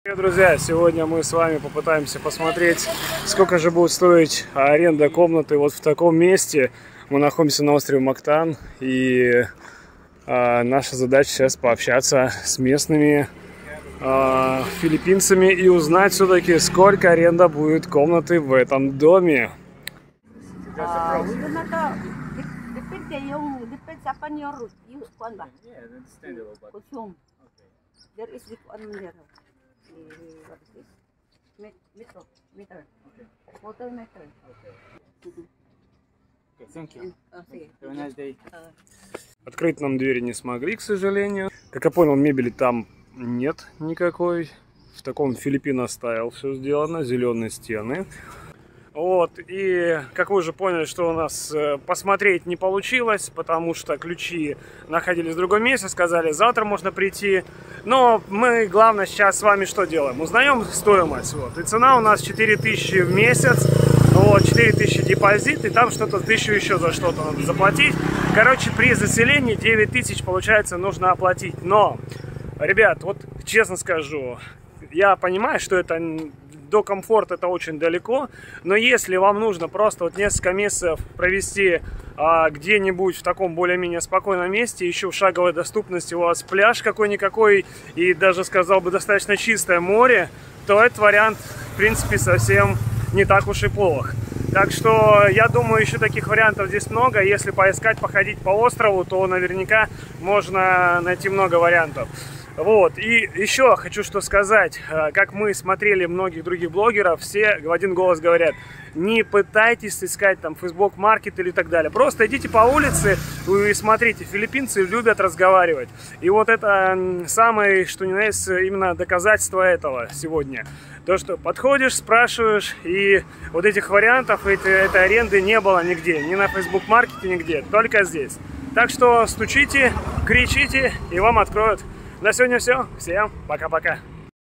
Снимаю дом возле моря за 4000 ? Остров Мактан. Сколько стоит аренда жилья в Себу
Бывает ли дешёвое жилье на берегу моря? смотрите это видео. Мы находим простые, но очень дешёвые варианты для аренды.